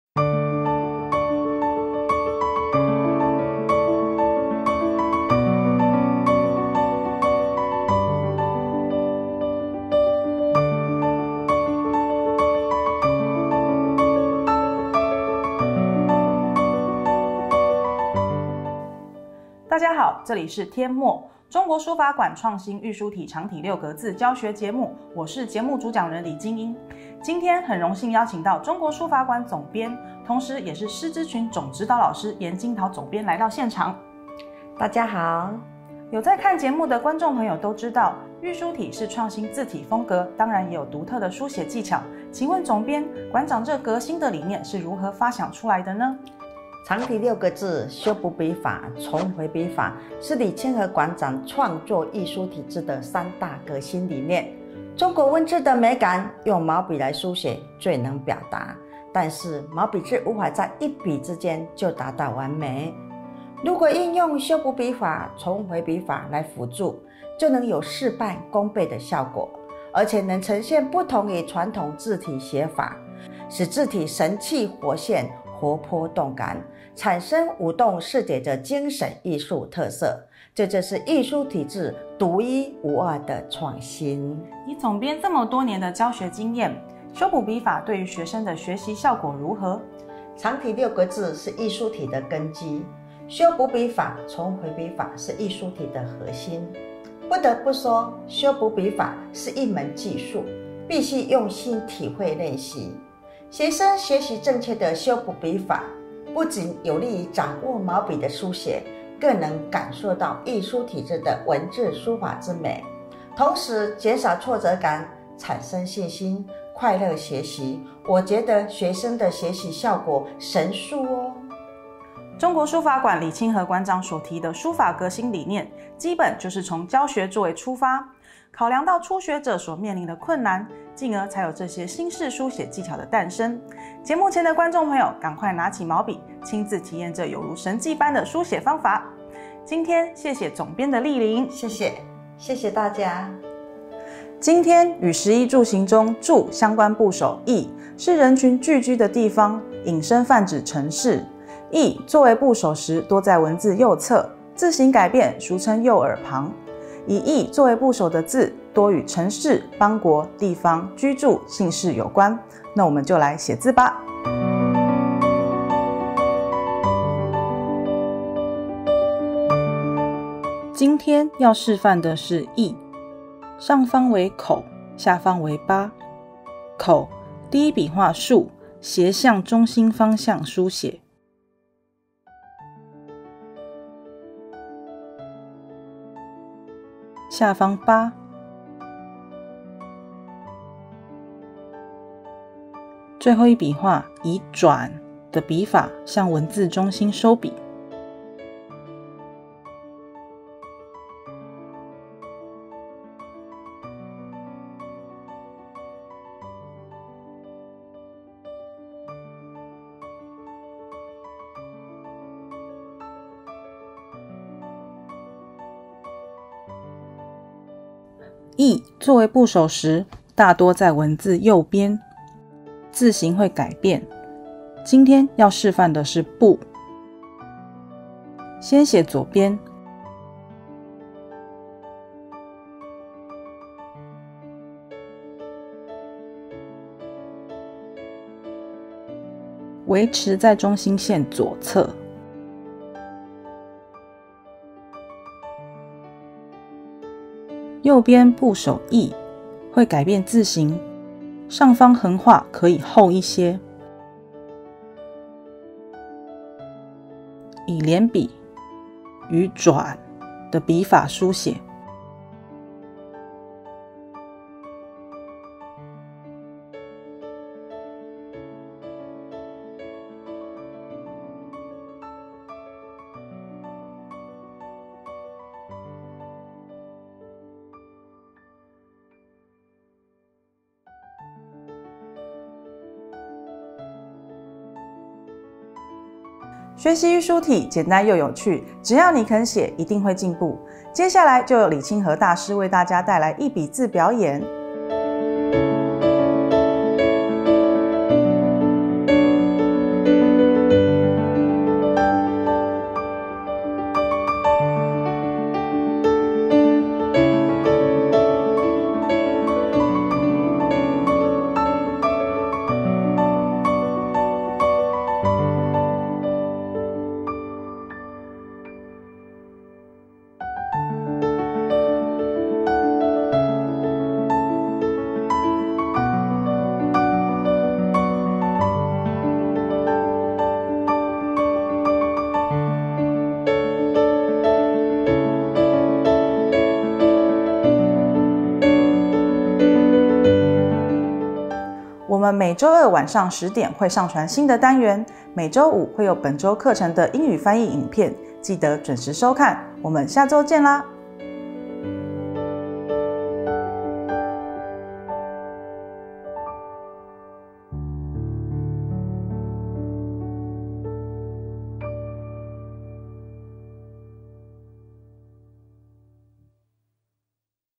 【天墨】中國書法館 創新「御書體」長體六格字書法教學節目。【第五十四堂課：與「住」有關的部首 - 邑、部】
【天墨書法教學節目 - 優質節目 創造優質社會 】